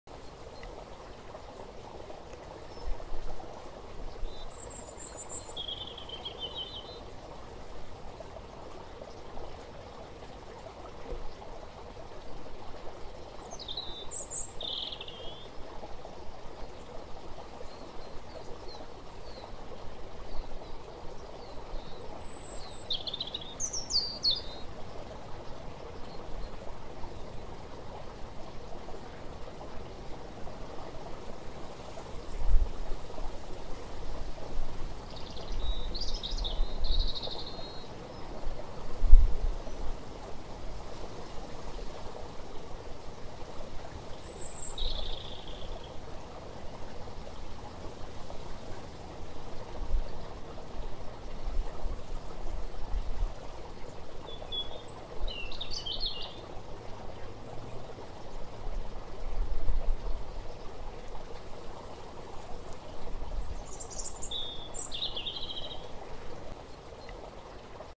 Bee Hawkmoth
Broad bordered bee hawkmoth filmed SW France with Sony RX10 Mk3.
Some video filmed at 1000 fps then slowed 40 times.